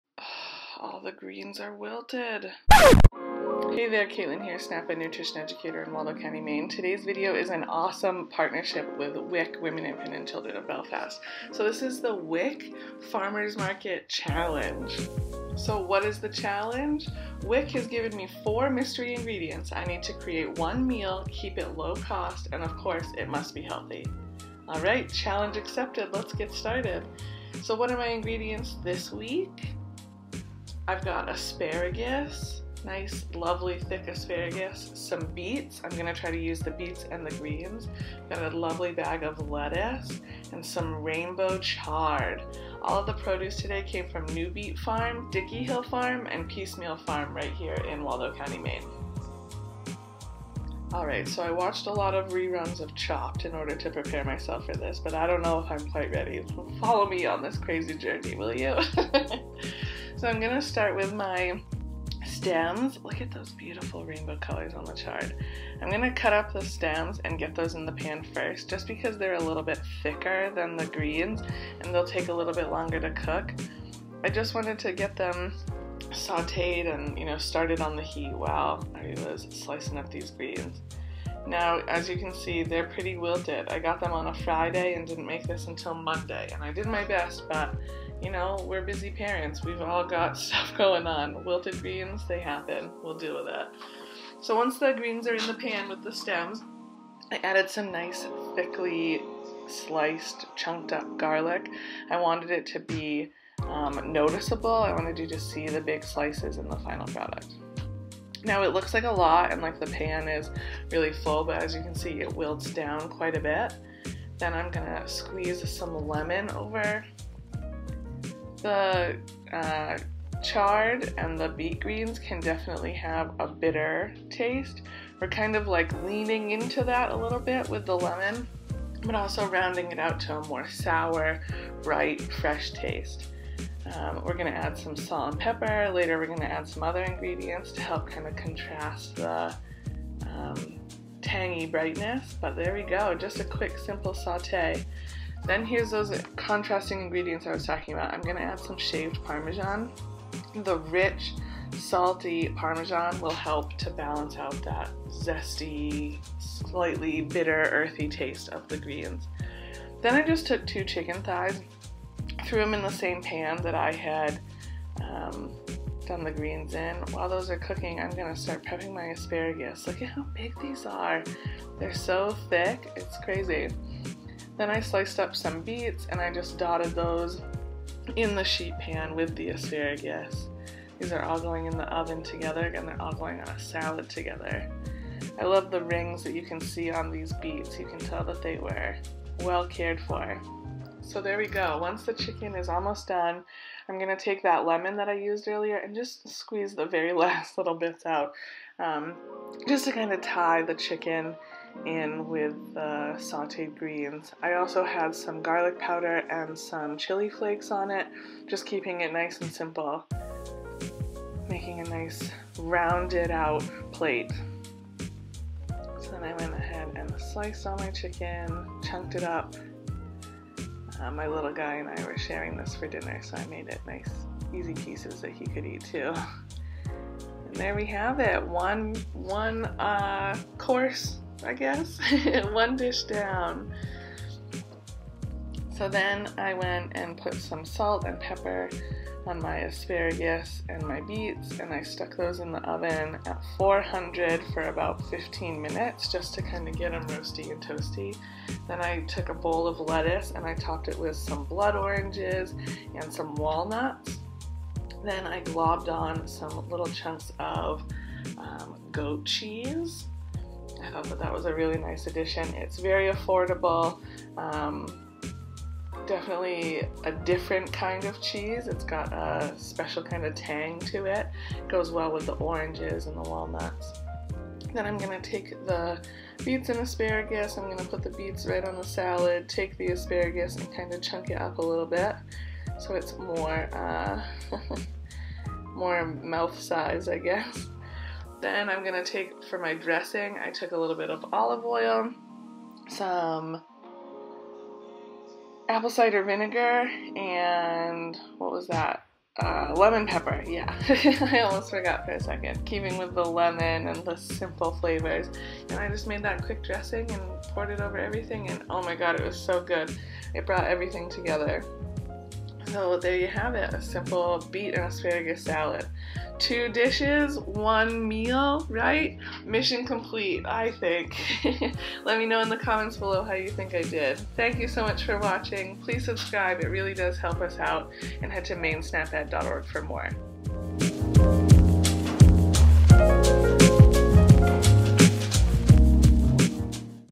WIC Farmers Market Challenge: Greens
The local WIC office has challenged me to take four mystery ingredients from the local Farmers Market and turn them into a healthy, affordable meal. This month, we tried some not-so-popular greens and made a veggie filled meal for two. Come check it out!